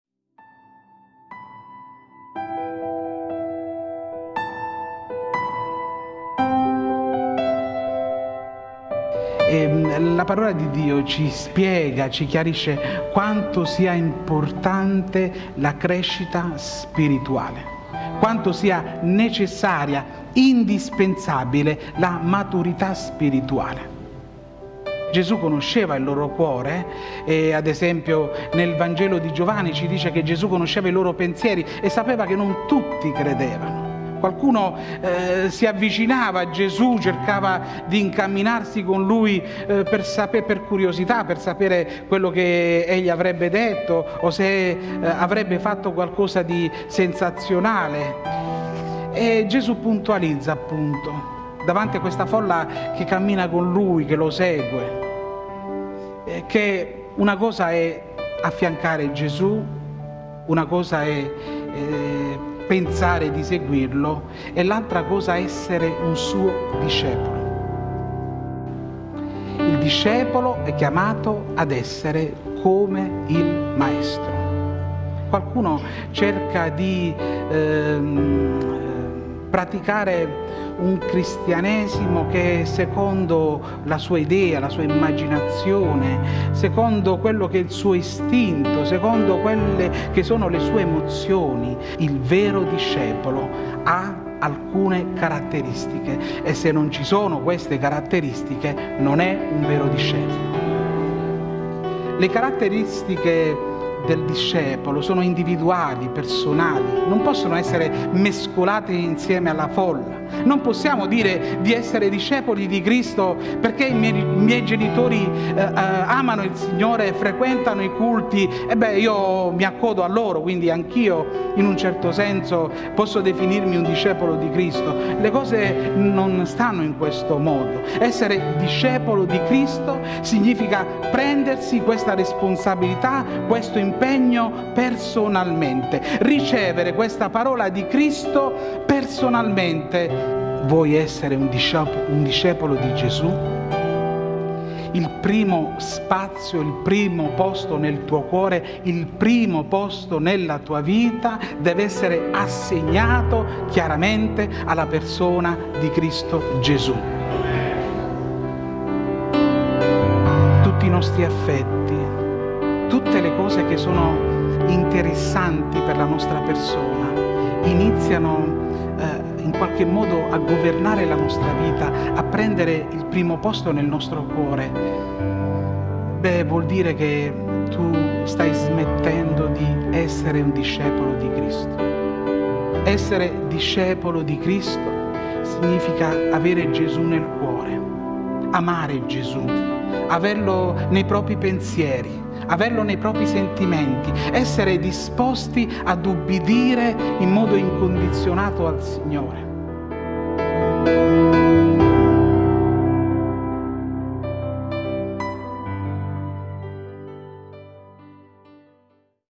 "Il vero discepolo di Cristo" - Luca 14:25-35 - Brevi Riflessioni - Daniele Ramunno
La parola di Dio ci spiega quanto sia importante la crescita spirituale, quanto sia necessaria la maturità spirituale.
Gesù conosceva il loro cuore. Ad esempio, il Vangelo di Giovanni ci dice che Gesù conosceva i loro pensieri e sapeva che non tutti credevano.
Qualcuno si avvicinava a Gesù e cercava di incamminarsi con Lui per curiosità, per sapere quello che Egli avrebbe detto o fatto, e Gesù puntualizza davanti a questa folla che Lo segue che una cosa è affiancare Gesù e pensare di seguirLo,
e l'altra cosa è essere un Suo discepolo.
Il discepolo è chiamato ad essere come il maestro.
Qualcuno cerca di praticare un cristianesimo che è secondo la sua immaginazione secondo quello che è il suo istinto, le sue emozioni.
Il vero discepolo ha alcune caratteristiche e se non ci sono non è un vero discepolo.
Le caratteristiche del discepolo sono individuali, non possono essere mescolate insieme alla folla. Non possiamo dire di essere discepoli di Cristo perché i miei genitori
amano il Signore e frequentano i culti e io mi accodo a loro.
Le cose non stanno in questo modo: essere un discepolo di Cristo significa prendersi questa responsabilità, questo impegno personalmente, ricevere questa parola di Cristo personalmente.
Vuoi essere un discepolo di Gesù?
Il primo spazio, il primo posto nel tuo cuore e nella tua vita deve essere assegnato chiaramente alla persona di Cristo Gesù.
Se tutti i nostri affetti, tutte le cose che sono interessanti per la nostra persona iniziano a governare la nostra vita, a prendere il primo posto nel nostro cuore, vuol dire che tu stai smettendo di essere un discepolo di Cristo.
Essere discepolo di Cristo significa avere Gesù nel cuore, amarLo, averLo nei propri pensieri, averLo nei propri sentimenti ed essere disposti ad ubbidire in modo incondizionato al Signore.